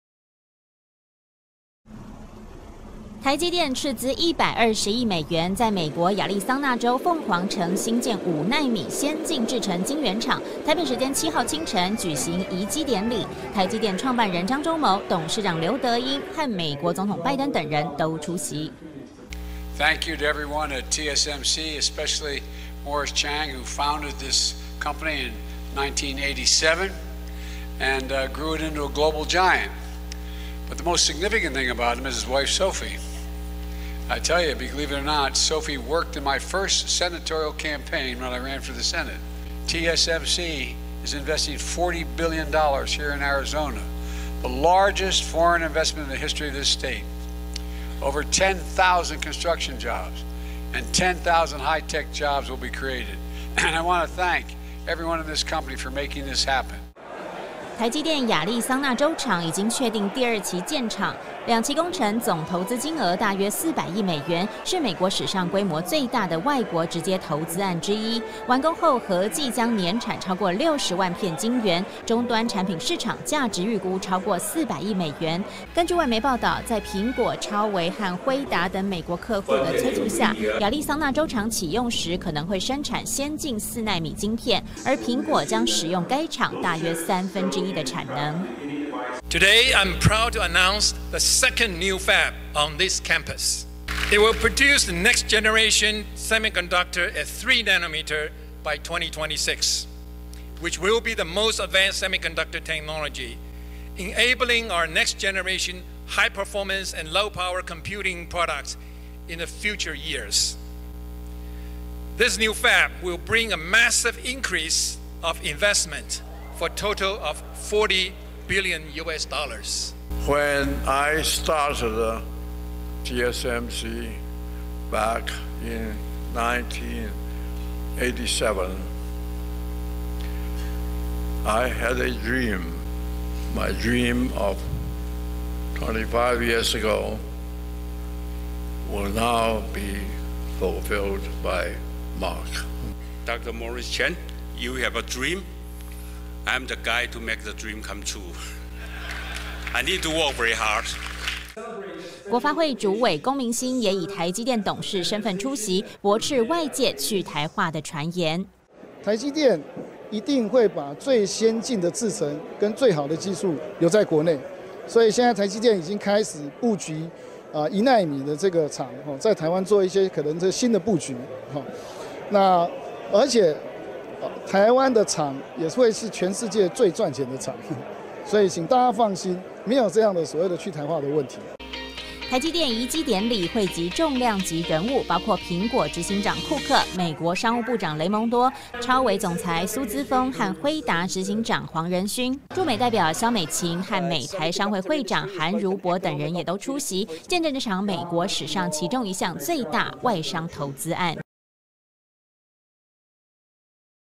台積電亞利桑那州廠移機典禮 美國總統親蒞致謝
台積電亞利桑那州鳳凰城晶圓廠移機典禮於近日盛大舉行，包括台積電創辦人張忠謀、董事長劉德音、美國總統拜登(Joe Biden)、商務部長雷蒙多(Gina Raimondo)及我國駐美代表蕭美琴大使等台美重要政商人士出席。拜登總統致詞感謝台積電，並強調400億美元投資總額是該州史上最大筆的外國直接投資，可望創造數萬個就業機會。我國國發會主委龔明鑫也以台積電董事的身分赴美出席，強調台積電仍將最先進的製程留在國內。